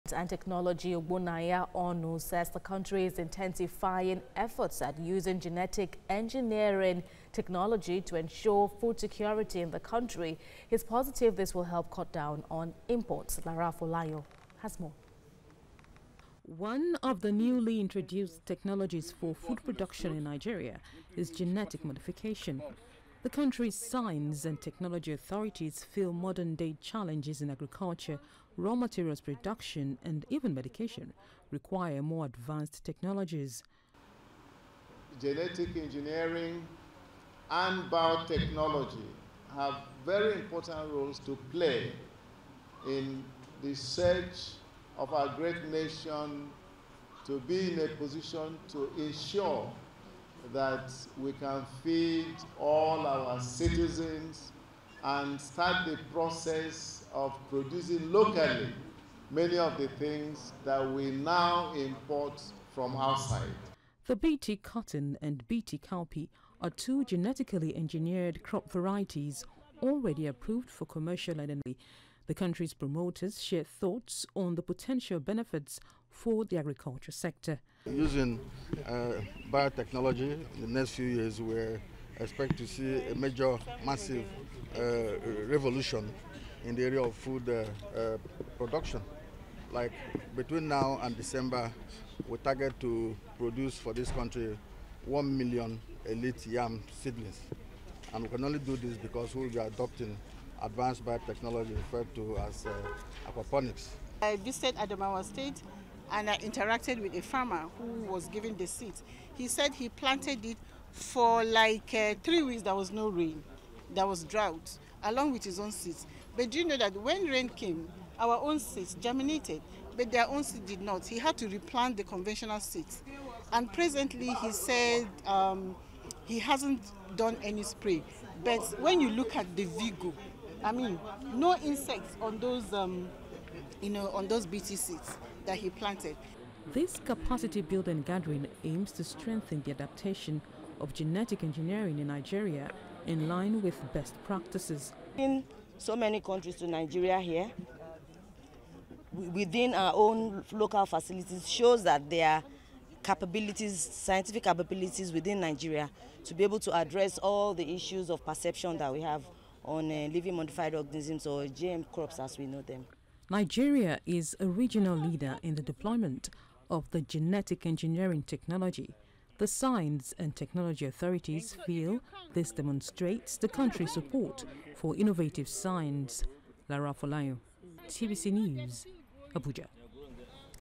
FG to invest in genetically engineered technology to ensure food security
The federal government said it will invest in genetically engineered technology to ensure food security in the country.

Lara Afolayan tells us more in this report.